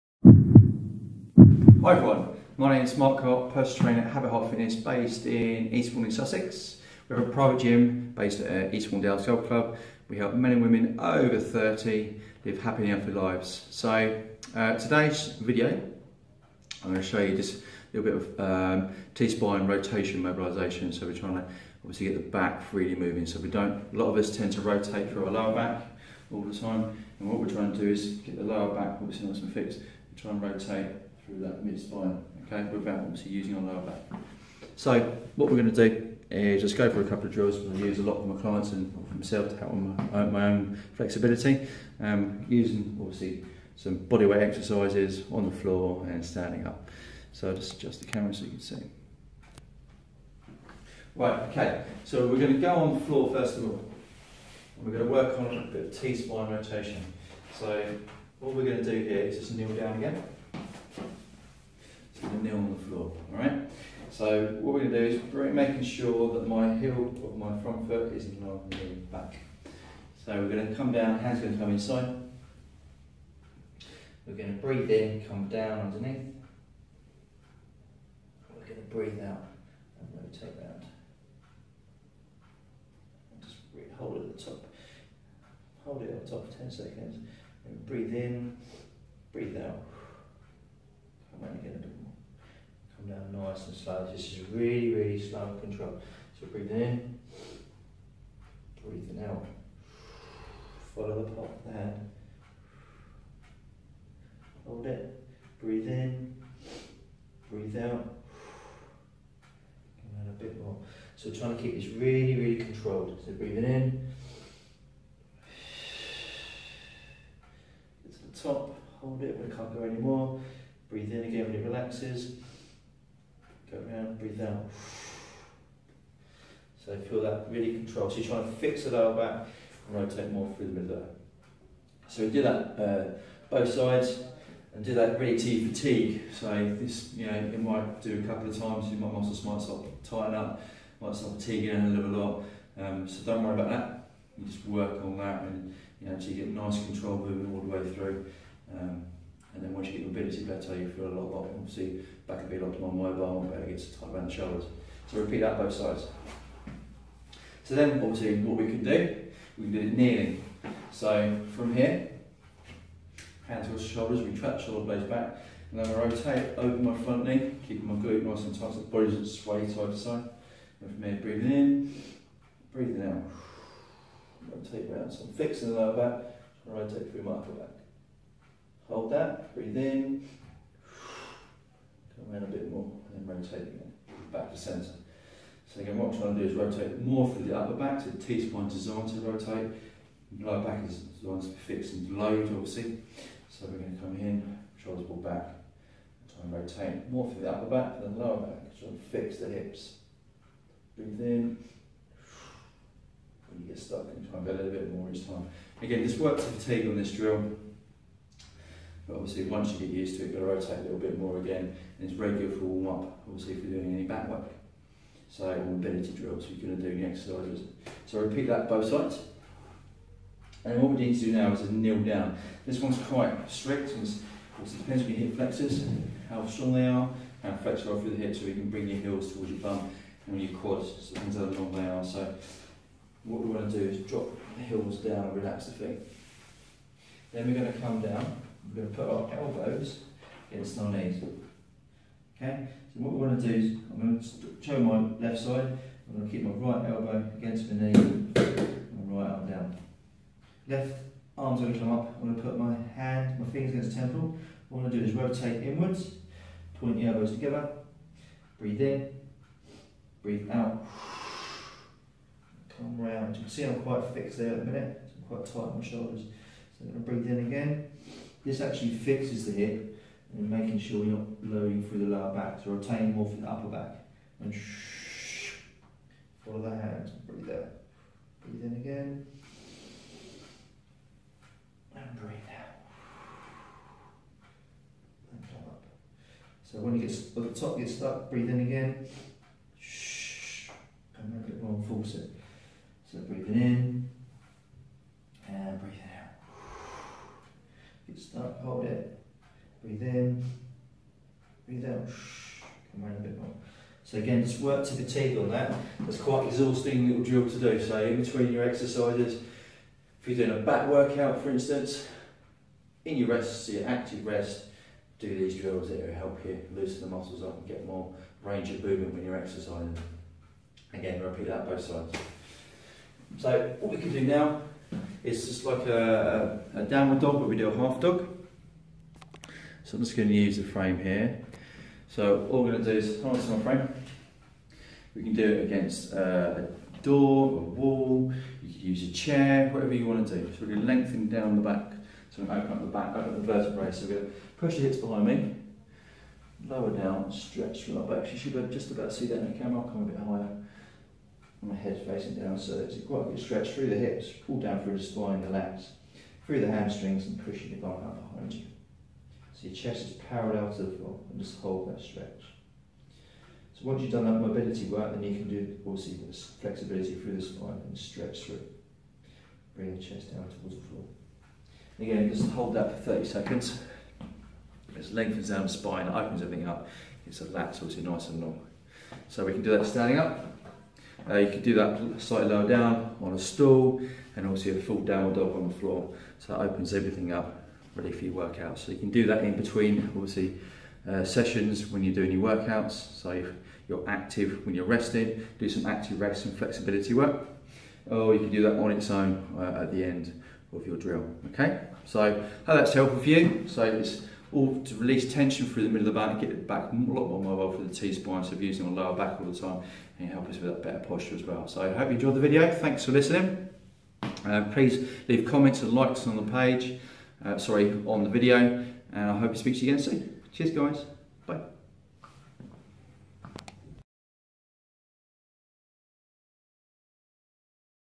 Tight Back - Back Flexibility Routine To Help Prevent Poor Posture And Improve Mobility
Tight Back  Back Flexibility Routine To Help Prevent Poor Posture And Improve Mobility For Men And Women Over 30
Mark Hart Personal Trainer at Have a Hart Fitness in Eastbourne shows you how to relieve stress in you back, improve your flexibility and become more mobile.
This video shows you various exercise using bands to help improve flexibility in you back, increase your back health and improve posture.
Tight T-spine or lack of flexibility can cause lower back pain and discomfort in your posture.
Daily back flexibility can help keep a healthy back and prevent stress in your daily lives.

New Videos the beginning of each month